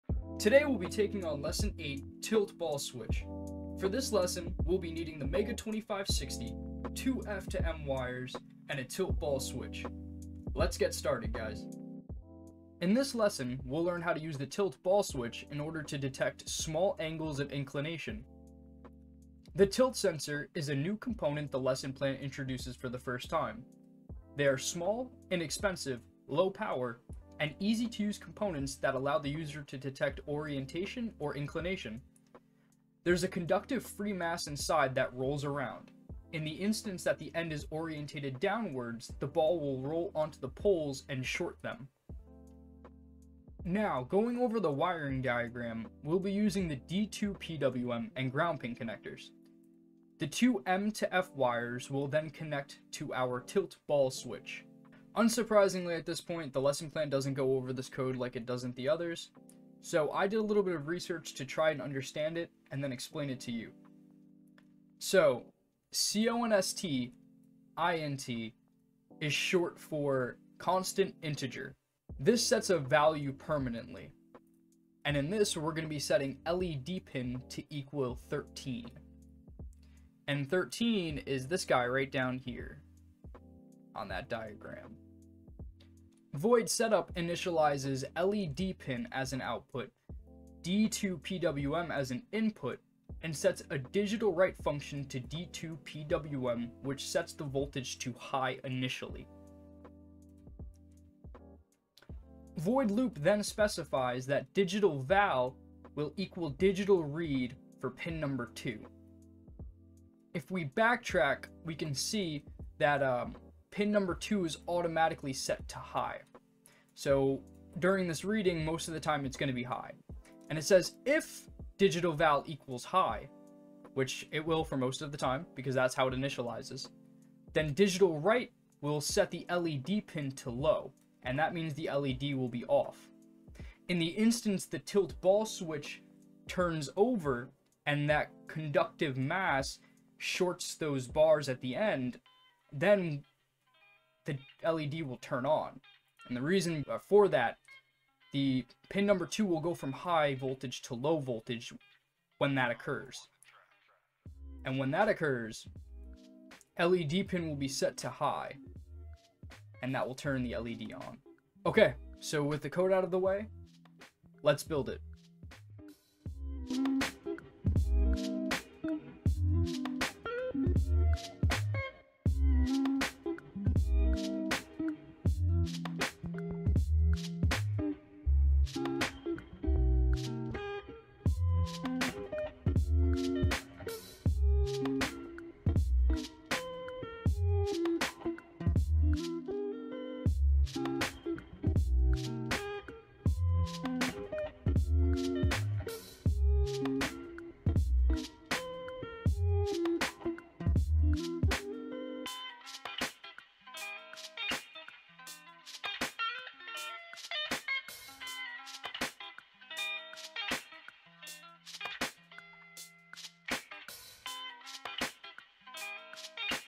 STARTER KIT TUTORIAL FOR MEGA 2560, Lesson 8 Tilt Ball Switch
THE MOST COMPLETE STARTER KIT
TUTORIAL FOR MEGA 2560!

Lesson 8 breakdown!

Music From:
Eric Godlow

Intro + components needed: (0:00)
Lesson Plan Intro: (0:12)
Lesson Plan - Tilt ball Switch: (0:18)
Lesson Plan - Wiring Diagram: (0:43)
Lesson Plan - Code: (0:54)
Building it!: (2:46)